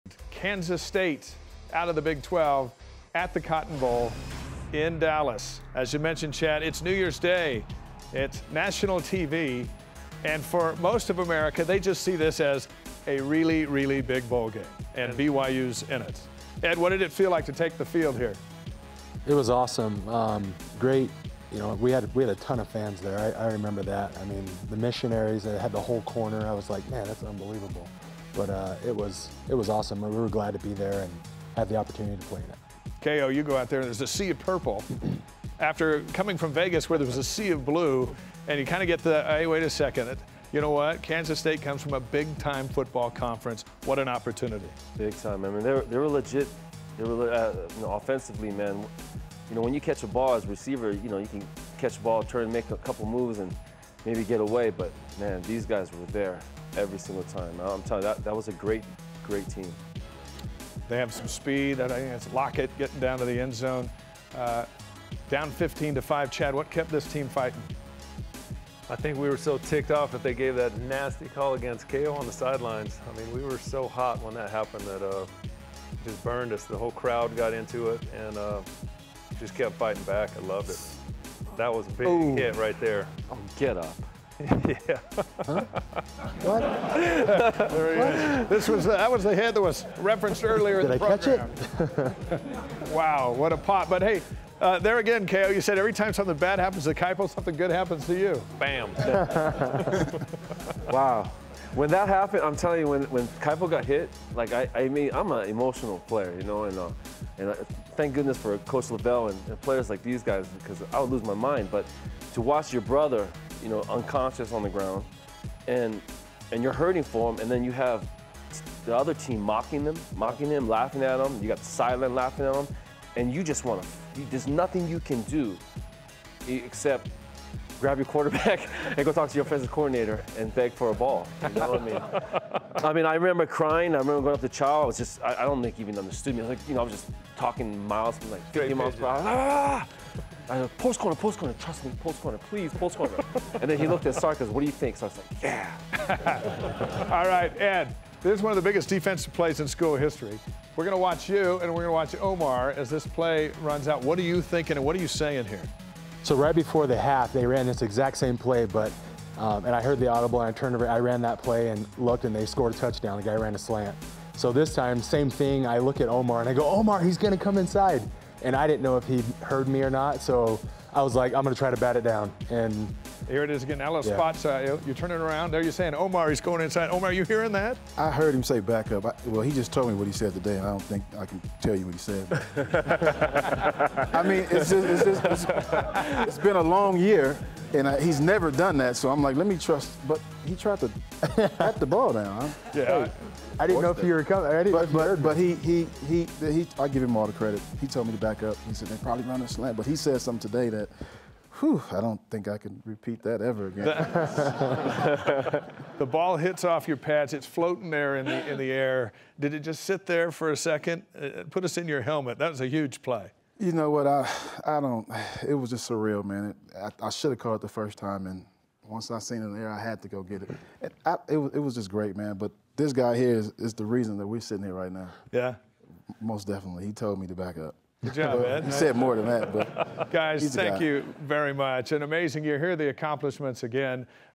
1996 Football Revisited: Cotton Bowl vs. #14 Kansas State
Members of the 1996 BYU Football team relive their epic Cotton Bowl victory over #14 Kansas State.